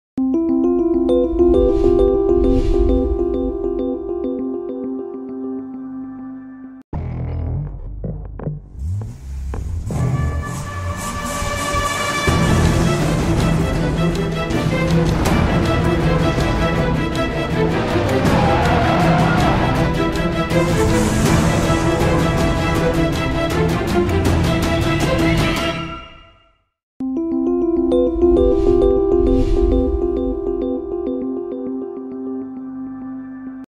RESERVOIR - Introducing NEW GT Tour I Jura Watches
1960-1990: it was during these years that so many legendary car races took place, forging the very essence of the RESERVOIR GT Tour Collection. This collection incarnates these values of precision, high performance and determination.